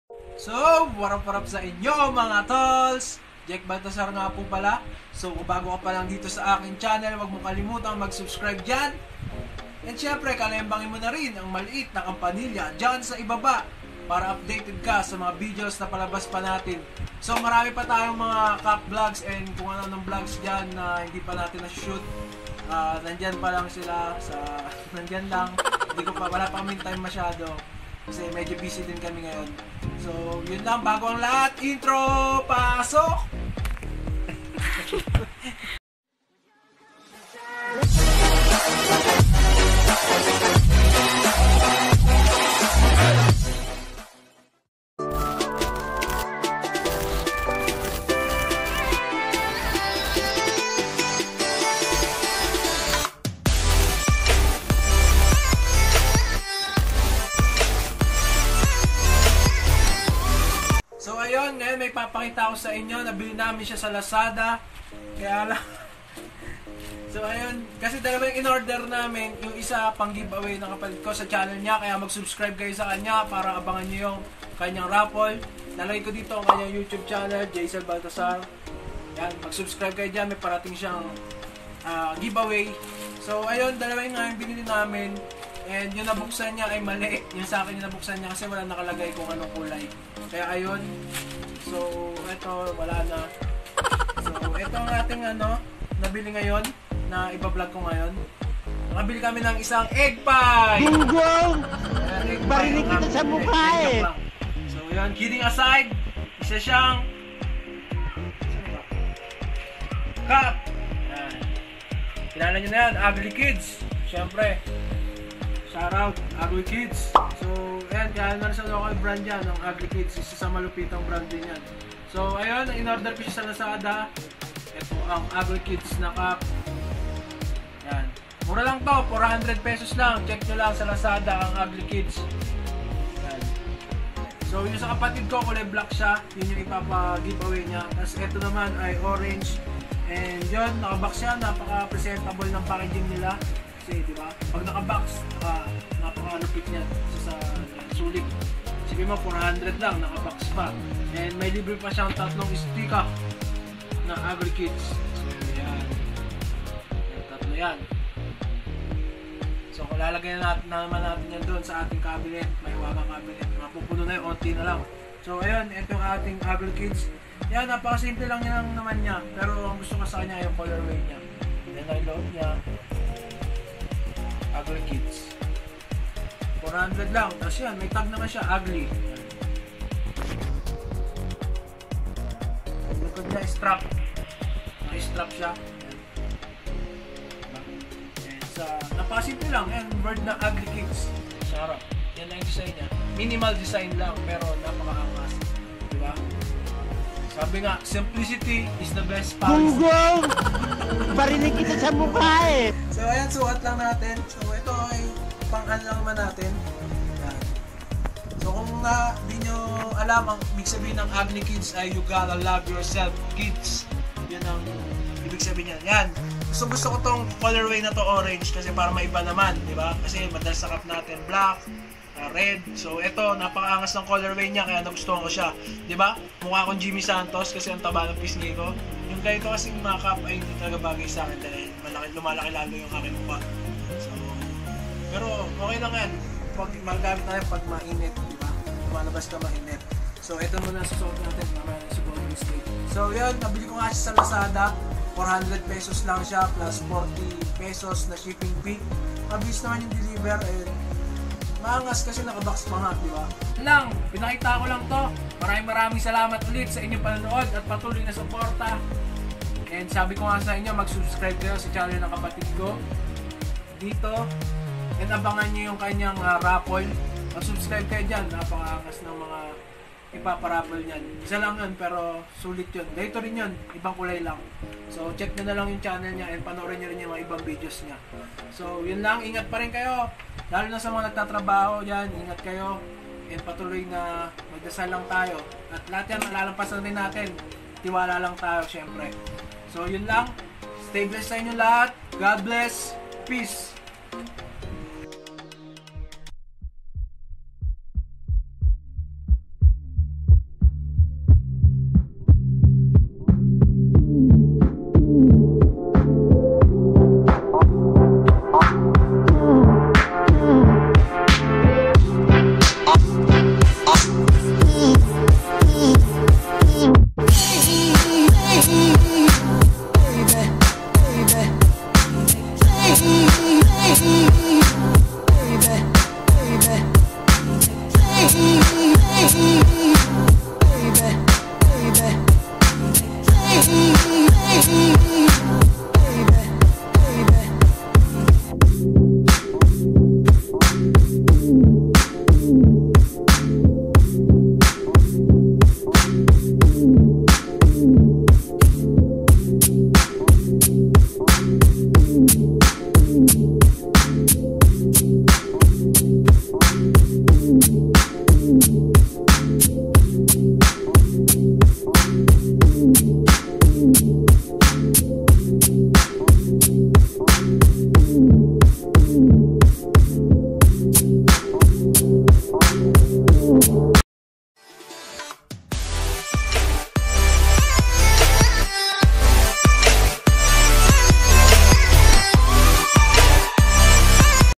UGLYKIDS CAP!
Uglykids Cap! Naorder ko sa Lazada! 400 pesos lang.. kilala na din sa Local to!

ingat kayo palagi.. Godbless at salamat sa lahat ng Supporta nyo!

Musician: Jef

Musician: @iksonofficial

Musician: Jef